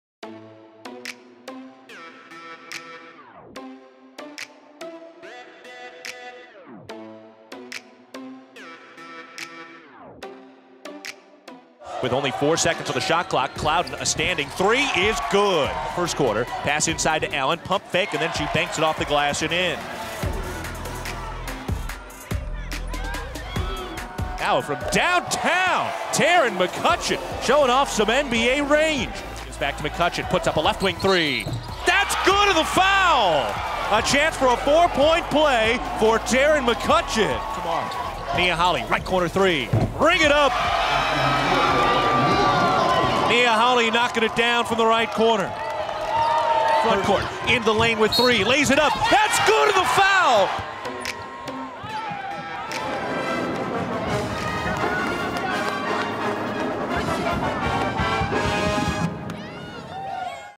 Michigan State vs Northwestern | Cinematic Highlight | W Basketball
Spartans defeat the Wildcats in the second round of the Big Ten Tournament in Indianapolis.